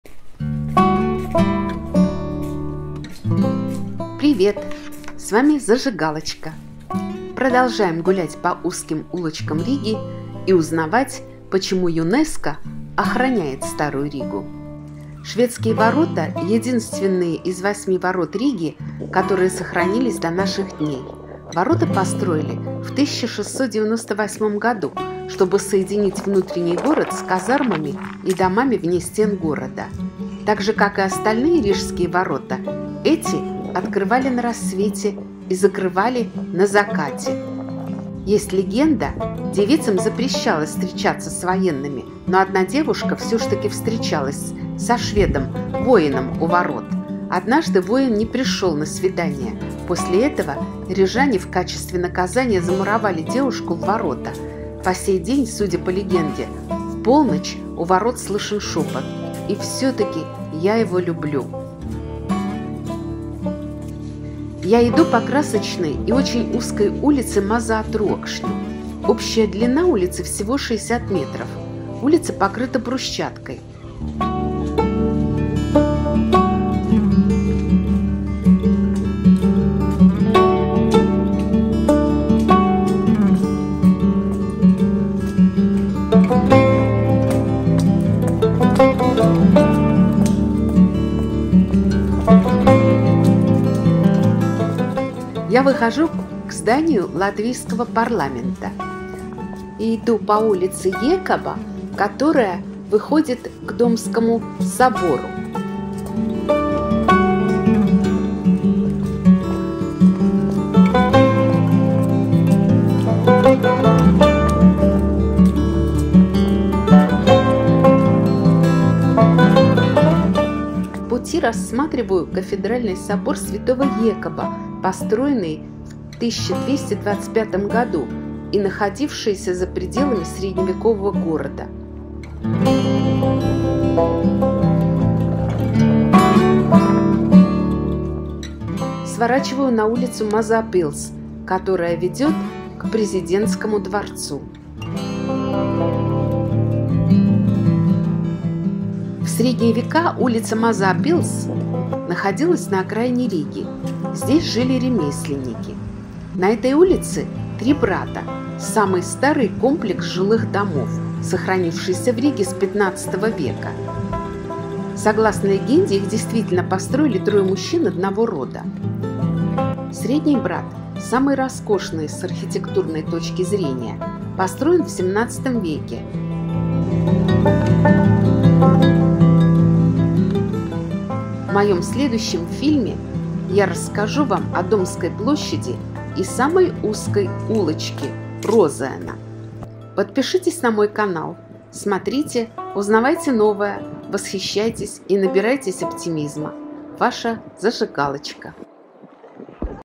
🔥ЮНЕСКO. Старая Рига 2. Шведские ворота и Три Брата.
🇱🇻 Привет! С вами 🔥Зажигалочка! Продолжаем гулять по узким улочкам Риги и узнавать почему ЮНЕСКО охраняет Старую Ригу.

Шведские ворота - единственные из 8 ворот Риги, которые сохранились до наших дней. Ворота построили в 1698 году, чтобы соединить внутренний город с казармами и домами вне стен города. Также как и остальные Рижские ворота, эти открывали на рассвете и закрывали на закате. Есть легенда – девицам запрещалось встречаться с военными, но одна девушка все-таки встречалась со шведским воином у ворот. Однажды воин не пришел на свидание. После этого рижане в качестве наказания замуровали девушку в ворота. По сей день, судя по легенде в полночь у ворот слышен шепот: «И все-таки я его люблю!». Услышать шепот могут только те, кто по-настоящему любил.

Я иду по красочной и очень узкой улице Маза Трокшню. Общая длина улицы всего 60 метров, она покрыта брусчаткой. Я выхожу к зданию латвийского парламента и иду по улице Екаба, которая выходит к Домскому собору.

По пути рассматриваю Кафедральный собор Святого Екаба, построенный в 1225 году и находившегося за пределами средневекового города, а потом сворачиваю на улицу Маза Пилс, которая ведет к президентскому дворцу.

В средние века улица Маза Пилс (Малая Замковая) находилась на окраине Риги. Здесь жили ремесленники.

На этой улице "Три брата" – самый старый комплекс жилых домов, сохранившийся в Риге с XV века. Свое название тесно прижавшиеся друг к другу средневековые здания получили очень давно – согласно легенде, их действительно построили трое мужчин одного рода.

Дом слева самый старый из "братьев", построен в 15 веке и имеет аскетическую архитектуру. Дом состоял из одного большого помещения, которое одновременно использовалось и как ремесленная мастерская, и лавка, и место для проживания людей.

Средний "брат" – самый роскошный с архитектурной точки зрения. Построен в 17 веке. В отличие от "старшего" брата, в доме был просторный зал с большими окнами, а жилые помещения располагались со стороны двора.

Третий "брат" построен во второй половине 17 века. На каждом его этаже располагались небольшие квартирки. Отличительной чертой дома является маска на фасаде, которая по замыслу его владельца должна была защищать дом от злых духов.

Исторический центр - средневековый старый город Риги, это древнейшая часть Риги. Старый город внесен в Список всемирного наследия ЮНЕСКО в 1997 году.

В моем следующем фильме я расскажу о Домской площади и самой узкой улочке Розэна.

Смотрите, узнавайте новое, восхищайтесь и набирайтесь оптимизма. Ваша 🔥Зажигалочка! 🥰